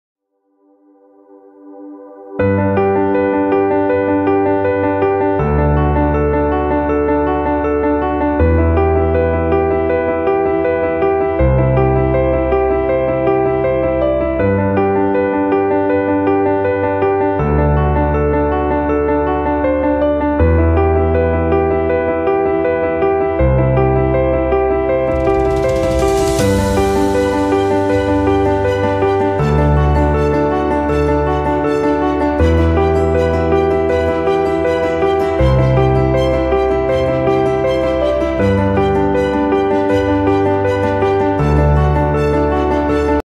ORICO USB C Docking Station, | Amazon Essentials
6 in 1 USB C Hub with 4K HDMI, 3 USB-A 3.0, USB-C 3.0, 100W PD Charging - Compact and High-Speed USB C Multiport Adapter for MacBook Air Pro, and Other Type C Devices this is a great addition to your busy lifestyle to order click the